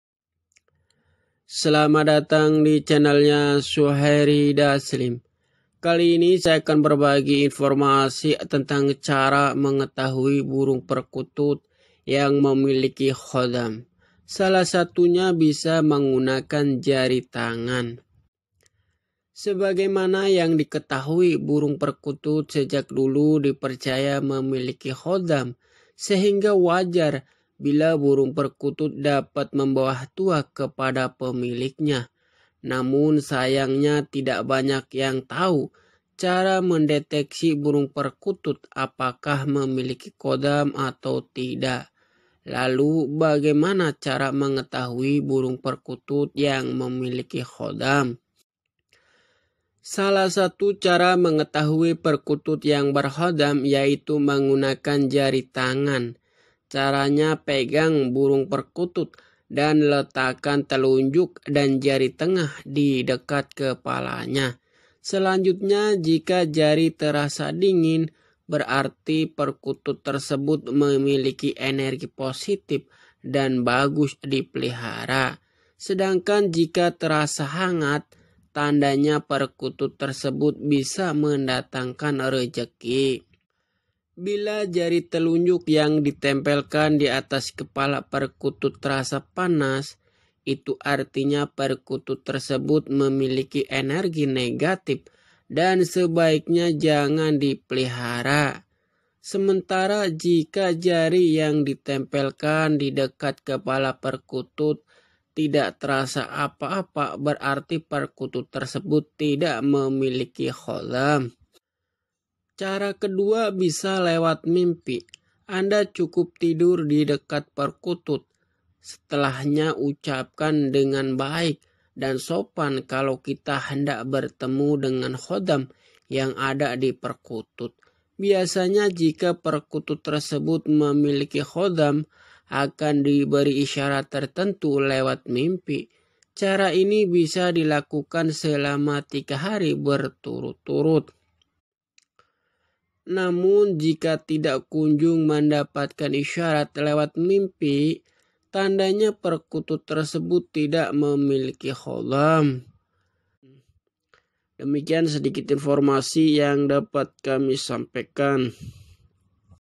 Cara Mudah Tes Perkutut Berkhodam Dengan Jari Tangan
Selamat datang di channelnya Suheri Daslim Klampok Wanasari Brebes

Sebagaimana yang diketahui burung perkutut sejak dulu dipercaya memiliki khodam sehingga wajar bila burung perkutut dapat membawa tuah kepada pemiliknya,

Namun sayangnya, tidak banyak yang tahu cara mendeteksi burung perkutut apakah memiliki khodam atau tidak,

Lalu bagaimana cara mengetahui burung perkutut yang memiliki khodam,